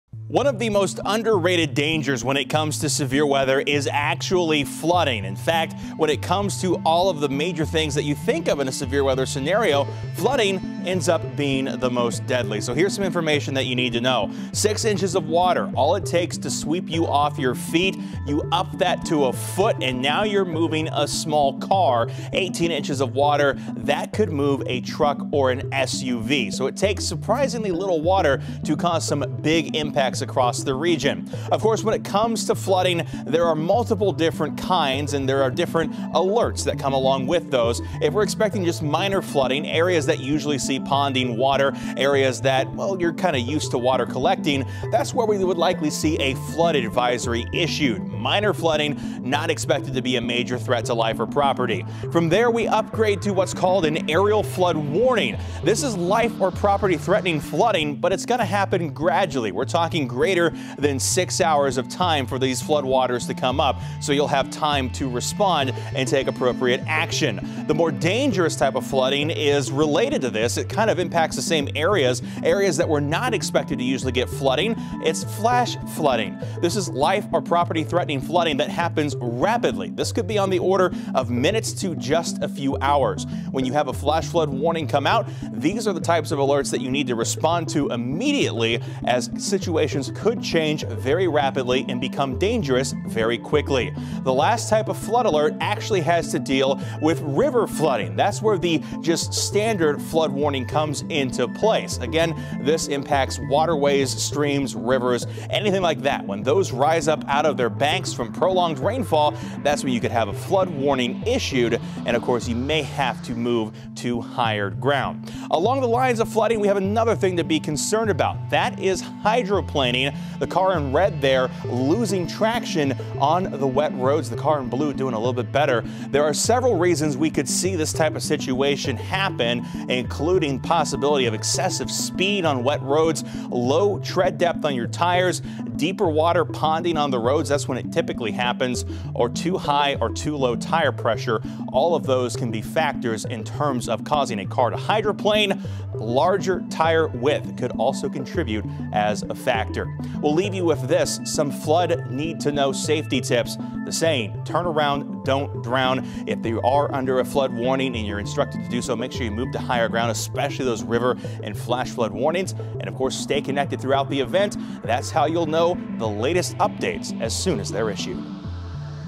Severe Weather Awareness Week: Flooding and Flood Safety
Flooding is the second leading cause of weather-related deaths following heat waves according, to the National Weather Service. Here's how to be ready!